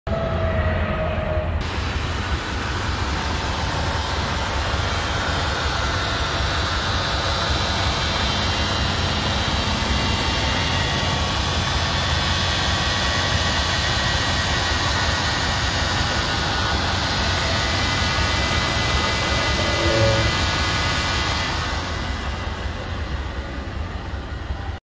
2000 Ford Excursion Limited SUV for sale at auction | bidding closes January 26, 2022
2000 Ford Excursion Limited  SUV
 Miles: 200,364 on odometer
 VIN: 1FMNU43L9YEB53560
Engine
 Displacement: 5.4L V8 SOHC 16V
 Cylinders: 8
 Fuel type: Gas
Transmission
 Automatic
 Four wheel drive
Interior
 AC, Heat
 Seat: Heated, Power driver
 Heated mirrors
 Power windows, Power locks
 Cruise control
Tires
 Size: 315/75R16
Missouri title